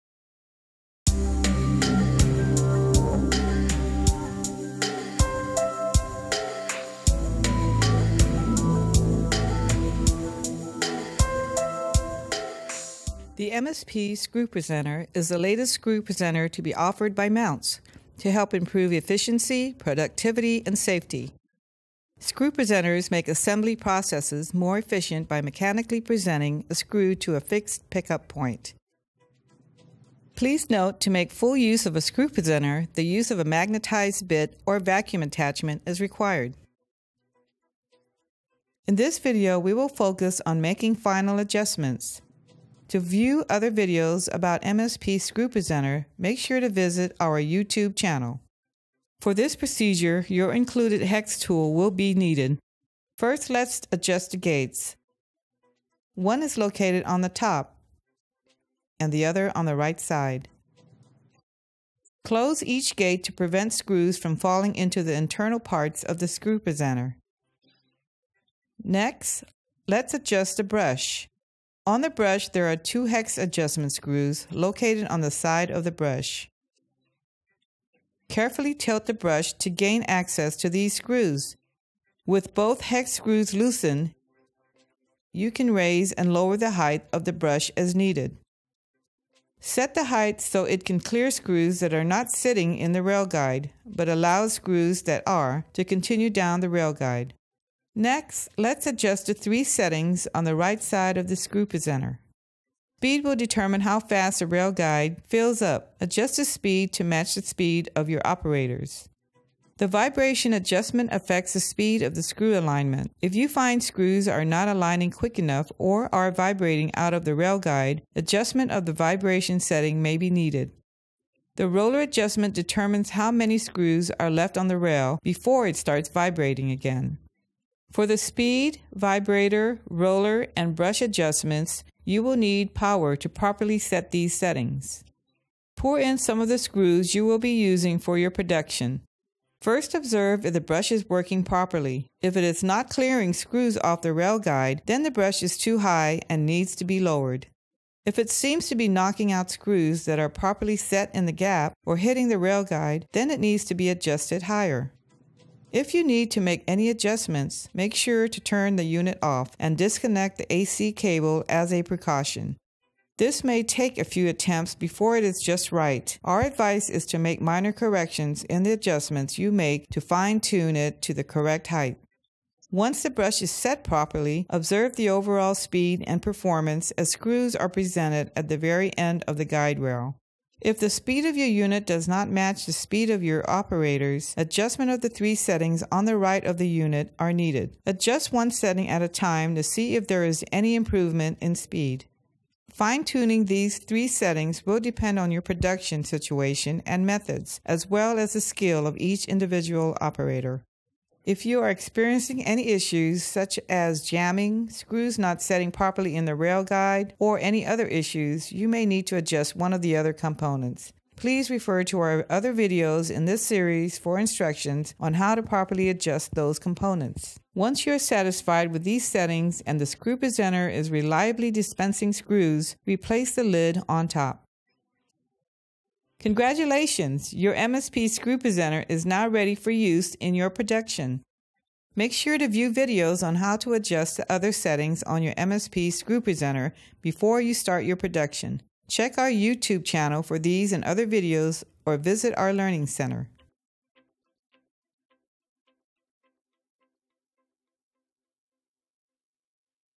Final Adjustments with the MSP Screw Presenter
Daily, assembly operators using power tools fumble around picking up screws from bins and slowly align the fasteners onto a bit. In an attempt to grab one fastener at time, several screws often stick to the operator's fingers. Using a screw presenter make the fastening process more efficient.

The video below provides a guide for making the final adjustments needed to operate the MSP screw presenter proficiently. These key adjustments help control the speed and efficiency of the screws being presented. If you require further technical support for making these adjustments, please call our support staff at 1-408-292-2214 and one of our representatives will be happy to assist you.